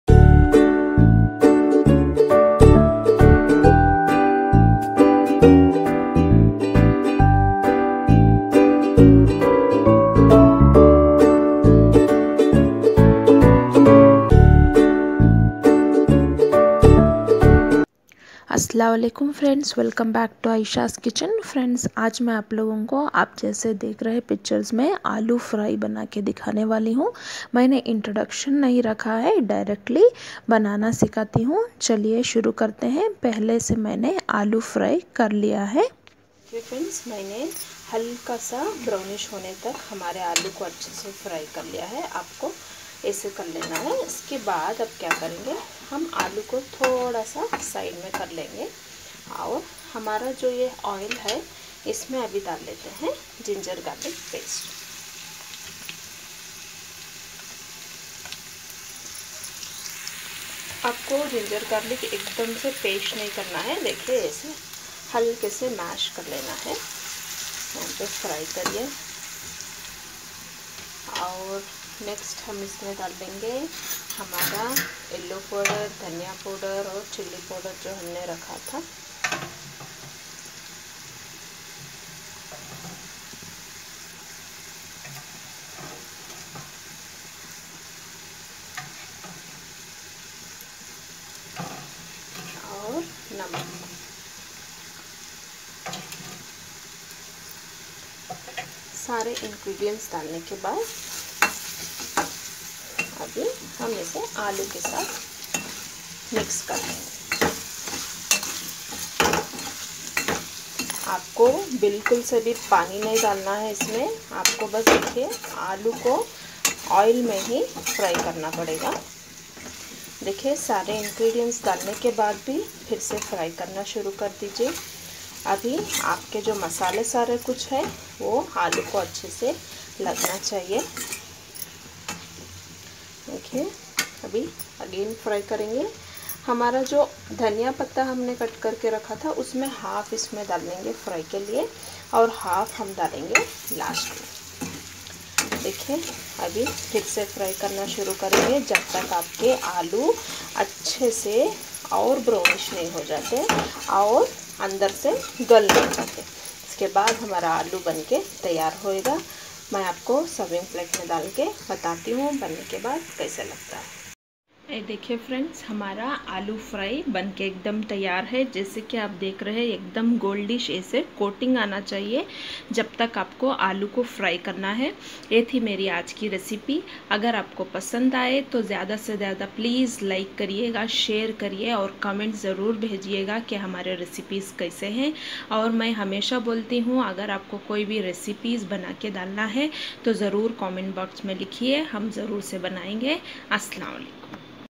Aalu fry/potato fry/Aalugadde fry
Assalamualikum friendz

Ayesha's kichen

Incridence

Aalu,ginger and garlic, coriande leafs, chilli powder, dhaniya power, yellow powder, salt,oil.

Assalamualikum